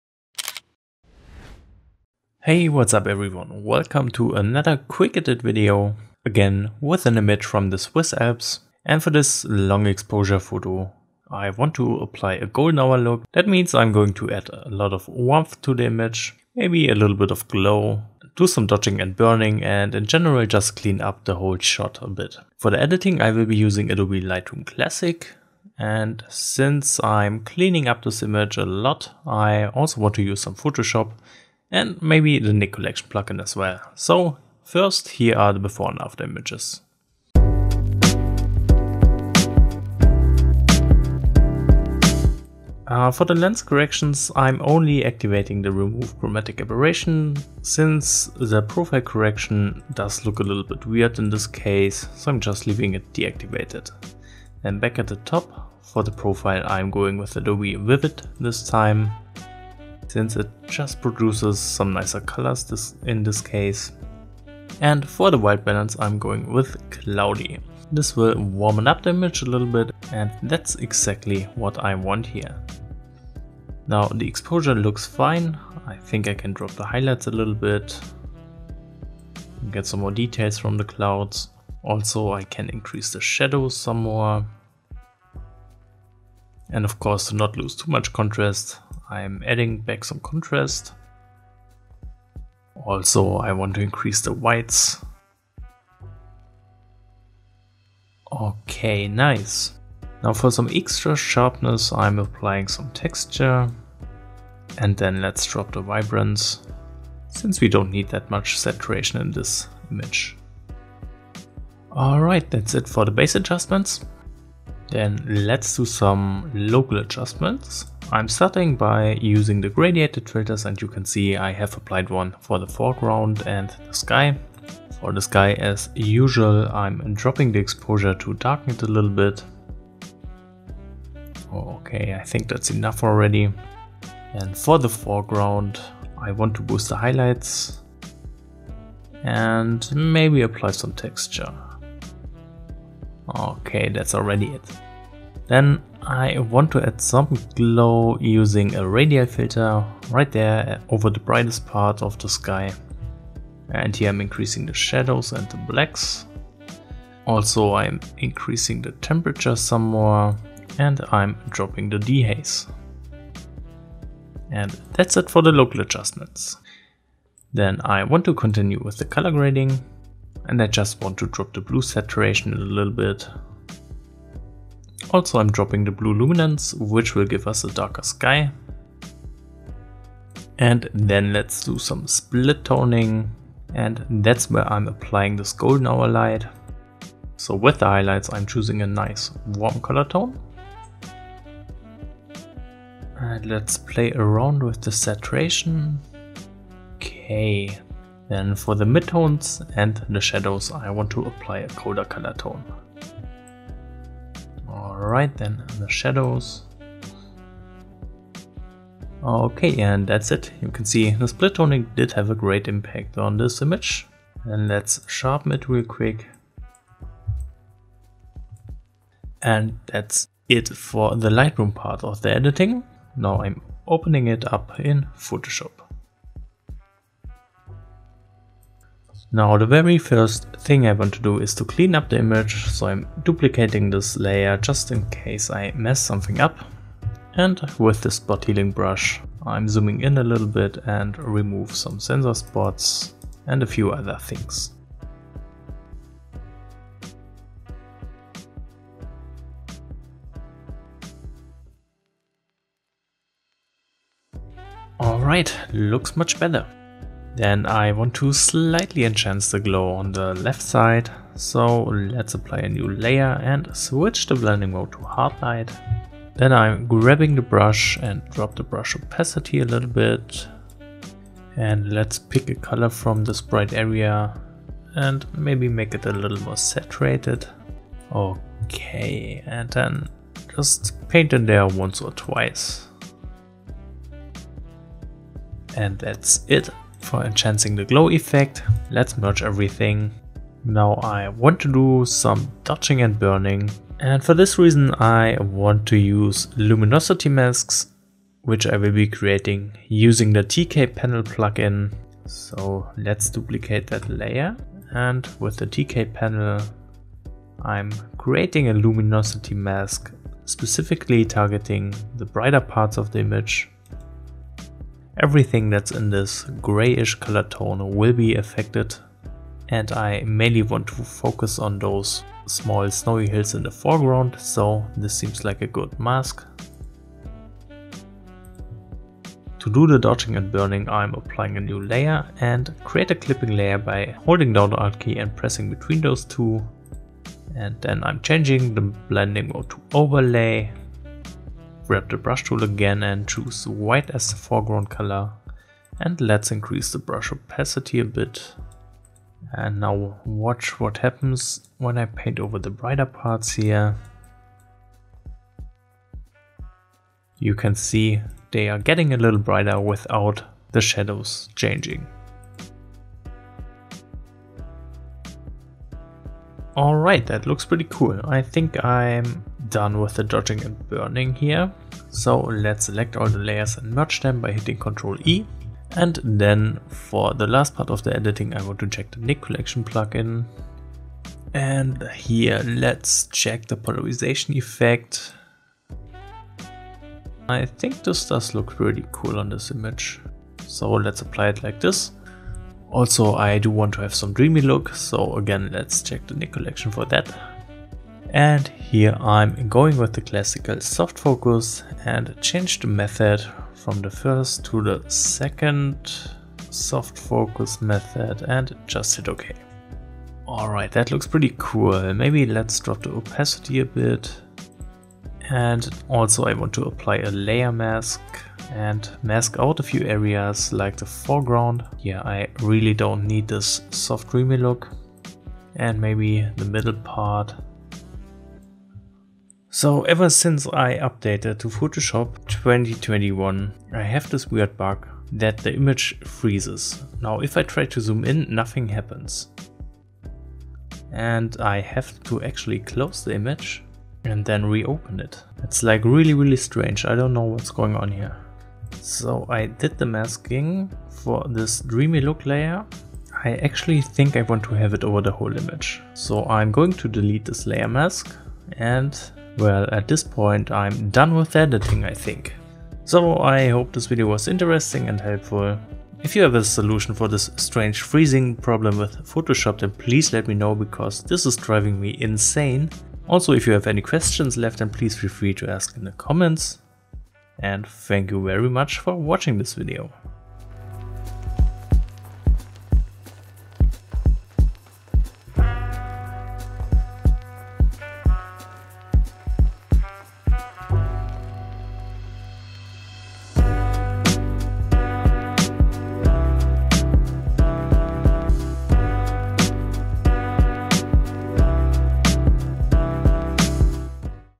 Dreamy Golden Light Effect with Adobe Lightroom & Photoshop | QE #289
In this quickedit video I‘m creating awesome, warm light using Adobe Lightroom Classic and Adobe Photoshop for the editing

0:00 *Intro*
My main goal here was to change the light and give it more of a golden hour look. Also, I wanted to add a subtle, dreamy style to the landscape and make the blue of the sky a lot darker to add more contrast. For the Editing I was using Adobe Lightroom Classic and Adobe Photoshop. The photo was captured on a Sony A7III at 32 mm, ISO 50, F/11 and 30 second exposure.

0:42 *1. Basic Adjustments*
I started by changing the camera profile to Adobe Vivid and the white balance to Cloudy. This will help warm up the photo a bit. To reveal details from the clouds I dropped the highlights, then increased the shadows and added some contrast.

1:55 *2. Local Adjustments*
To darken the sky, I dropped a graduated filter over it and reduced the exposure. Another graduated filter was used over the foreground. Here, I increased the texture and also added highlights. With a radial filter I added a glowing spot on the bright part on the left since I didn’t like how that area looked before. Yes, I know, a lot of people hate this glow, I just think it looks cool! :-)

2:47 *3. Color Grading*
First, I dropped the blue saturation and luminance (to make the sky darker). Then, With the split toning I added a strong golden color to the highlights and used a cold tone on the midtones and the shadows.

3:48 *4. Photoshop*
With the spot healing brush, I removed some of the grass in the foreground and a few sensor spots. Then, I added some more glow using a hard light layer and a warm, soft brush. Finally, I used the Nik Collection plug in with the polarization effect for enhanced colors and the classic soft focus for the dreamy look.